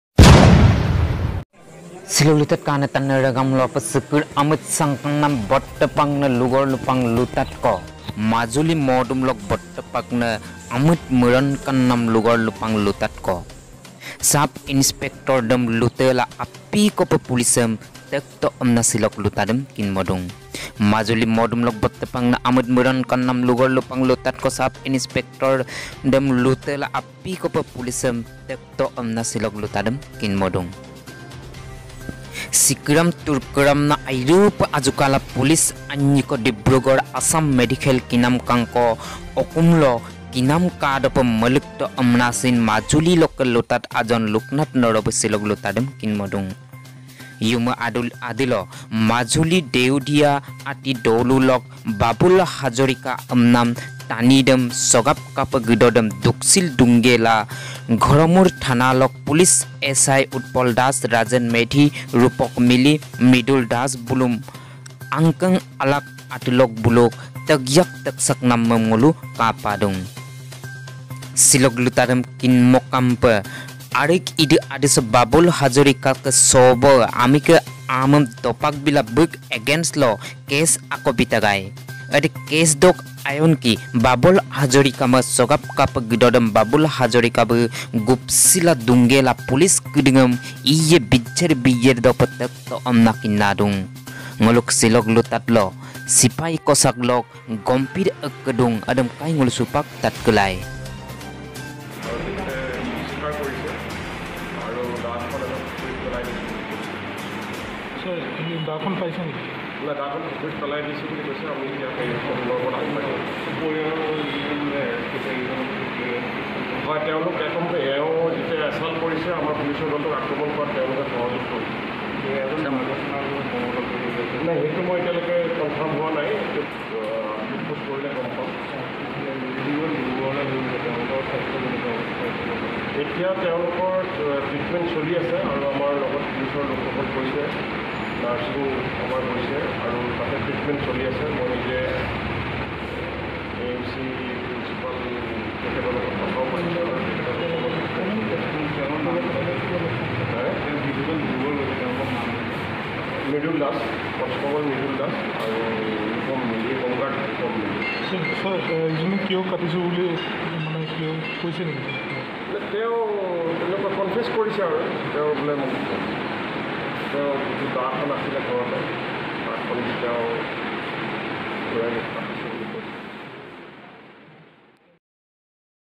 SILOK LUTAD#মাজুলীত উপ-পৰিদৰ্শক সহ চাৰিগৰাকী আৰক্ষীক দা-ৰে ঘপিয়াই আক্ৰমণ#গুৰুতৰভাৱে আহত দুজন
SILOK LUTAD#মাজুলীত উপ-পৰিদৰ্শক সহ চাৰিগৰাকী আৰক্ষীক দা-ৰে ঘপিয়াই আক্ৰণ#গুৰুতৰভাৱে আহত দুজন